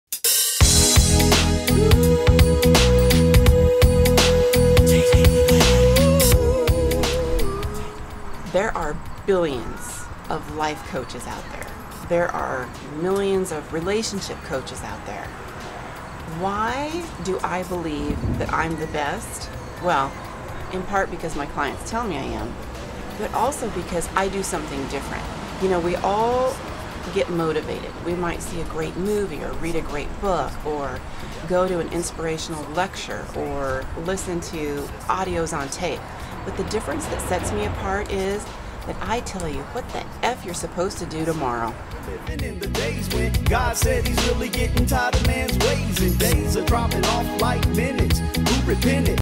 Dollar Coach Club - 2
What makes a good life coach?  Robin has the answer to this provocative question.